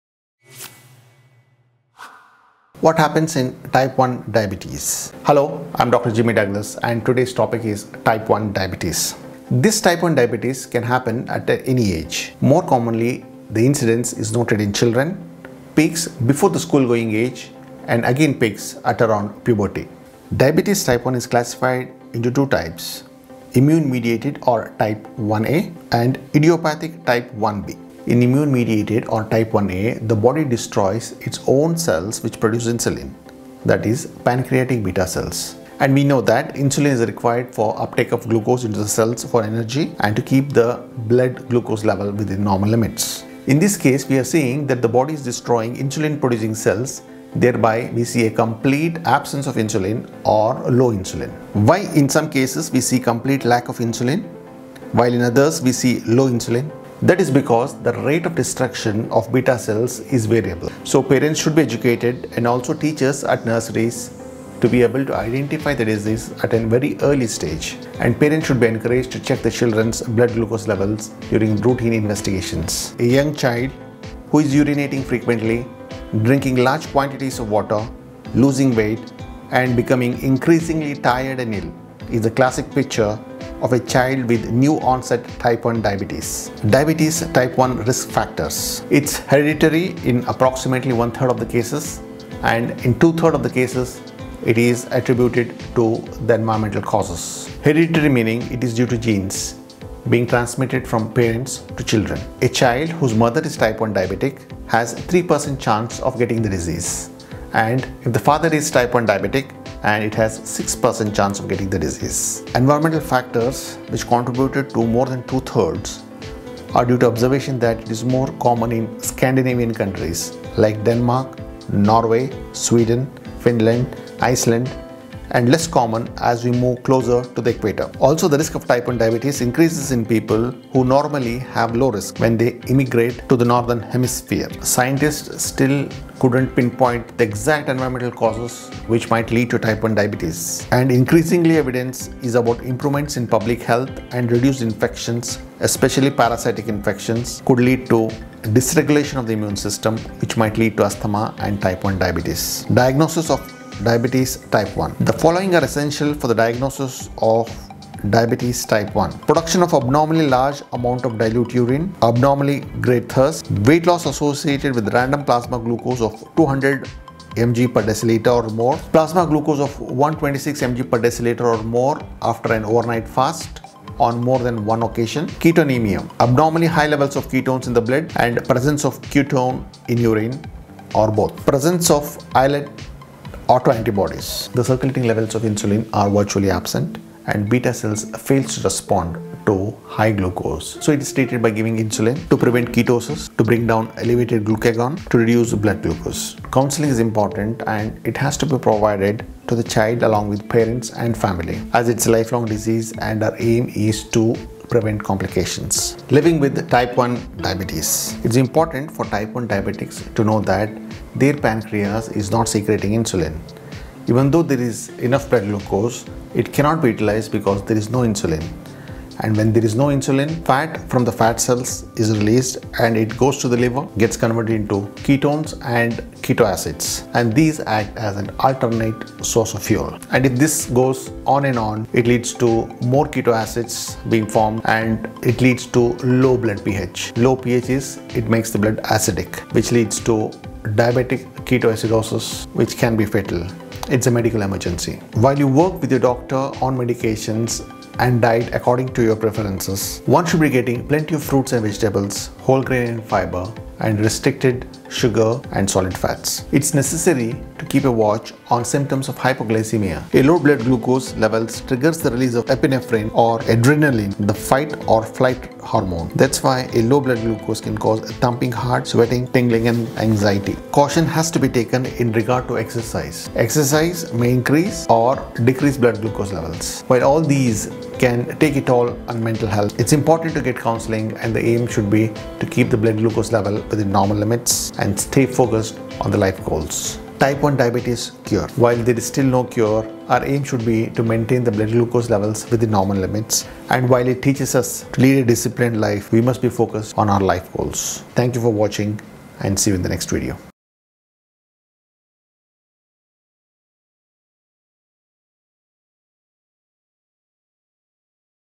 What Happens in Type 1 Diabetes ?
Learn what happens in Type 1 Diabetes, classification or types, symptoms, risk factors, diagnosis and living with type 1 diabetes

✅ Diabetes type 1 is classified into 2 types
Immune Mediated or Type 1A
Idiopathic Type 1B

1. Frequent Urination
2.Thirst
3. Hunger, which is constant
4. Weight loss, despite normal or increased appetite
5. Blurred Vision
6.  Hypotension
7. Fatigue

✅ Diabetes type 1 risk factors
It’s Hereditary and approximately one-third of disease susceptibility is due to genes and two-thirds to environmental factors.

✅ Diagnosis of Diabetes type 1
The following are essential for Diagnosis of Diabetes Type 1

▶️ Polyuria (Production of abnormally large volume of dilute urine),
▶️ Polydipsia (Abnormally great thirst) and
▶️ Weight Loss associated with random plasma glucose of 200 mg/dL or more
▶️ Plasma Glucose of 126 mg/dL or more after an overnight fast, on more than one occasion
▶️ Ketonemia (Abnormally high levels of ketones in the blood) and Ketonuria (Presence of ketones in Urine) or Both
▶️  Presence of Islet autoantibodies

✅ Living with Type 1 Diabetes
Patients must be made aware of the disease process and what happens if it is left untreated.
While it’s important for Diabetics Type 1 to know that , their pancreas is not secreting insulin, even though there is glucose, the cells cannot use glucose as there is no insulin, and when there is not enough insulin, fat is released from the fat cells and they go to liver and get converted into ketone and ketoacids, and they act as alternative source of fuel to the cells.

As more and more keto acids are formed, it makes the blood acidic . This condition is called DKA or Diabetic KetoAcidosis and it can be fatal. It is a Medical Emergency.

✅  Type 1 Diabetes Cure
While there is no cure . Researchers are relentlessly working on a number of strategies to replace b-cells in people with type 1 diabetes.
Aim should be to keep the blood sugars within normal limits.